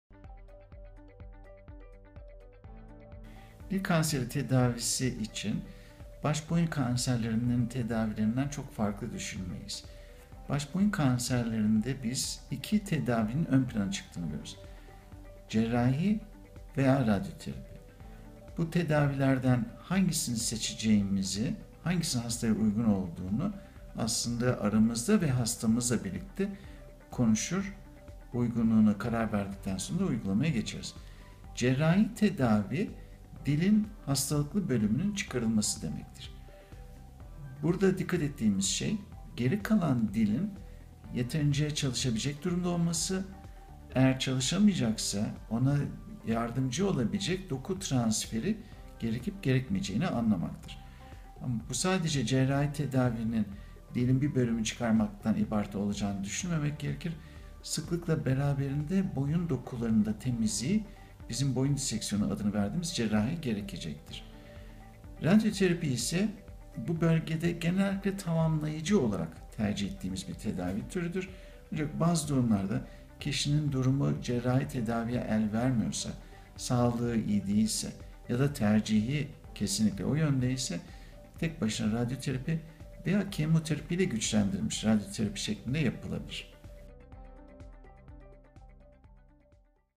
Dil kanserinin tedavisi nasıl yapılır? - Op. Dr. Mazhar Çelikoyar
Op. Dr. Mazhar Çelikoyar “Dil kanserinin tedavisi nasıl yapılır?” sorusunun yanıtını veriyor.

Dil kanserinin tedavisi için baş boyun kanserlerinin tedavilerinden çok farklı düşünülmelidir. Dil kanserlerinde iki tedavinin ön plana çıktığı görülür: cerrahi veya radyoterapi.

Bu tedavilerden hangisinin seçileceğine, hangisinin uygun olduğuna, hasta ile konuşarak birlikte karar verilir ve uygulamaya geçilir. Cerrahi tedavi dilin hastalıklı bölümünün çıkarılması demektir. Burada dikkat edilecek nokta, geride kalan dilin yeterince çalışabilecek durumda olması, eğer çalışamayacaksa ona yardımcı olabilecek doku transferinin gerekip gerekmediğini anlamaktadır. Cerrahi tedavinin sadece dilin bir bölümünün çıkarılması olarak düşünülmemeli, sıklıkla beraberinde, boyun dokularını da temizleme yani boyun diseksiyounu denen cerrahi de gerekmektedir. Radyoterapi ise bu bölgede genellikle tamamlayıcı olarak tercih edilen bir tedavi türüdür. Bazı hallerde kişinin durumu cerrahi tedaviye el vermiyorsa, sağlığı iyi değilse ya da tercihi kesinlikle o yöndeyse, tek başına radyoterapi veya kemoterapiyle güçlendirilmiş radyaterapi de yapılabilir.